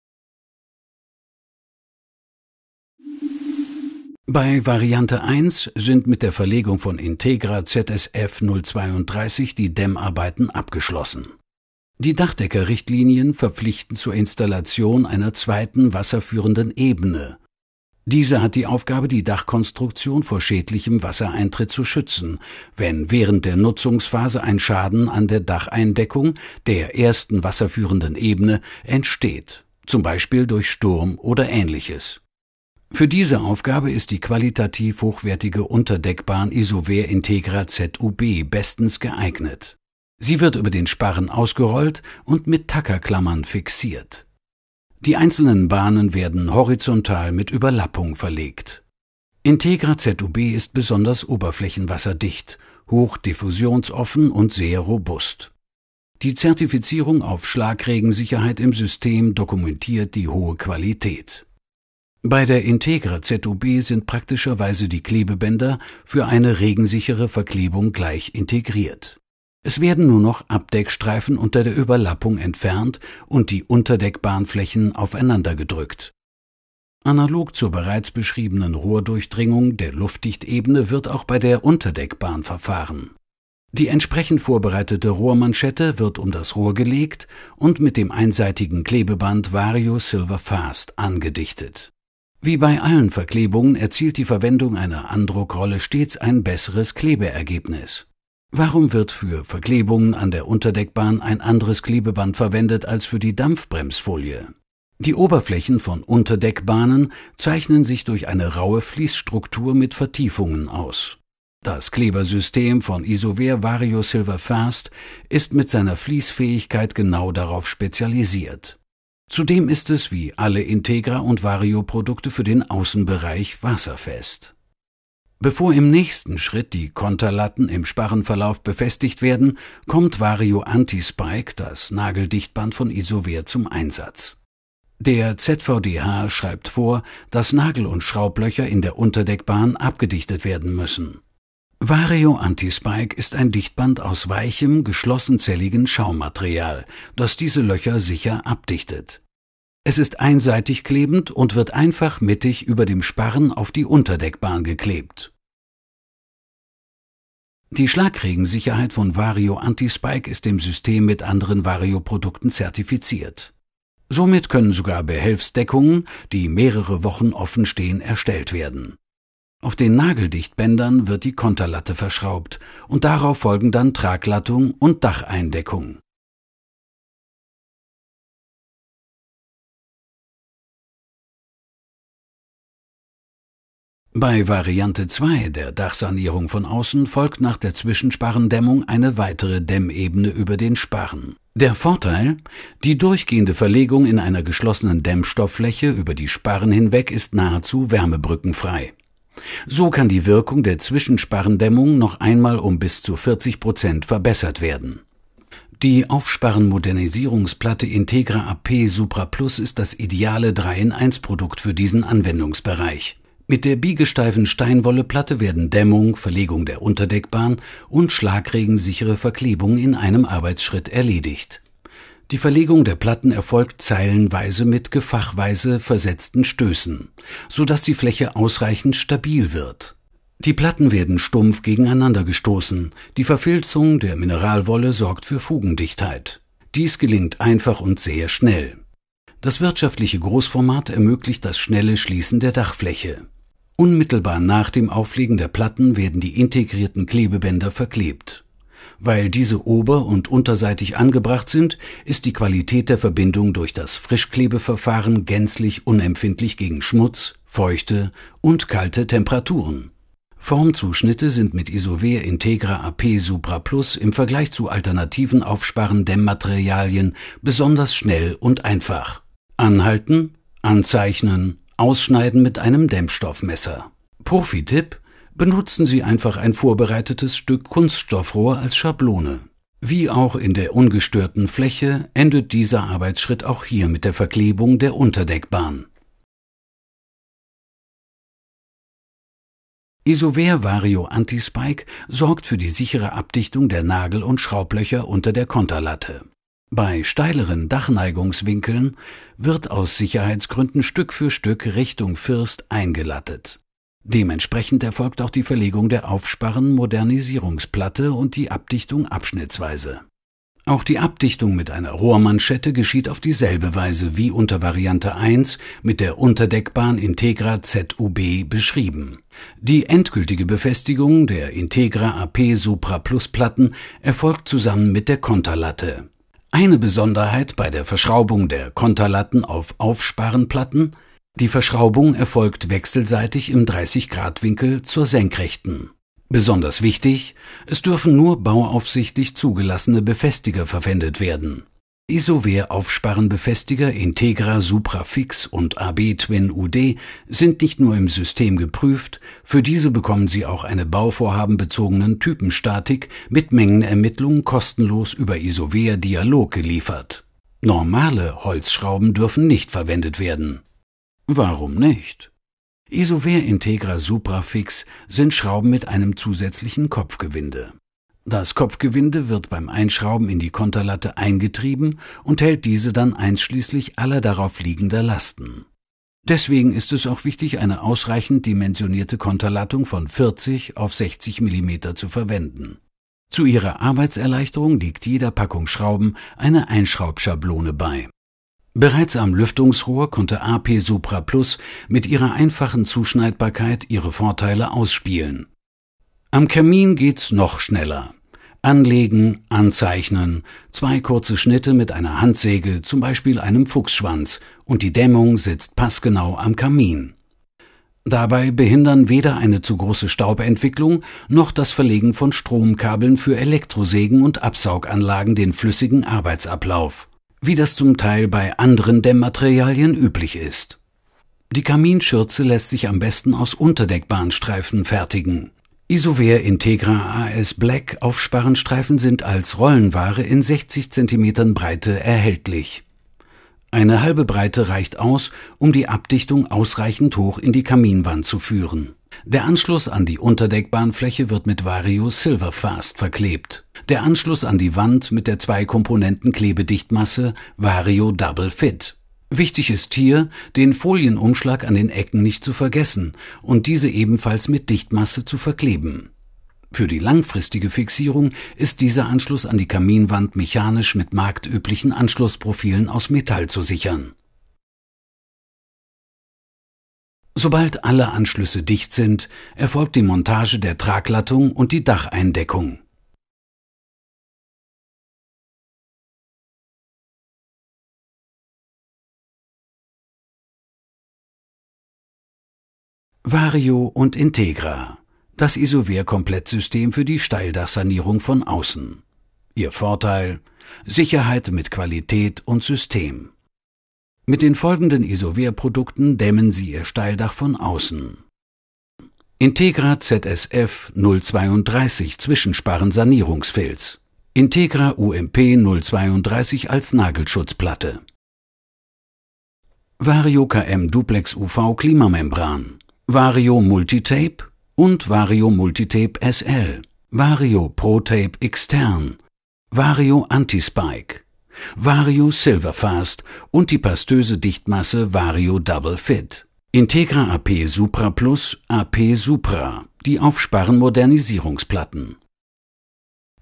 Dachsanierung Außen Profitipps Teil 4 | ISOVER
In den ISOVER Profitipps Videos Teil 1-4 erhalten Sie detaillierte Informationen über die Produkte sowie hilfreiche Tipps zur Verarbeitung.

Zum Überblick:
0:00 Variante 1
3:04 Variante 2
8:05 Vorstellung Komplettsystem

Der Inhalt dieses Films entspricht dem Stand unseres Wissens und unserer Erfahrung zum Zeitpunkt der Erstellung. Die Angaben stellen jedoch keine Garantie im Rechtssinne dar. Besondere Verhältnisse des Einzelfalls können nicht berücksichtigt werden.